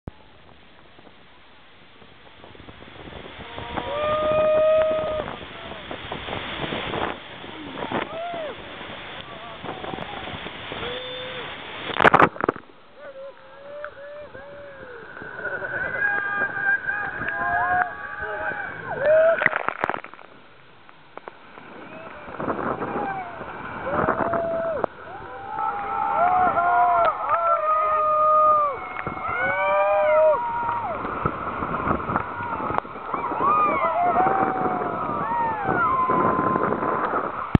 Riding Lava Falls with Wilderness River Adventures
River rafting on the Colorado river in the Grand Canyon. This Vid is of us on our last day rafting Lava Falls.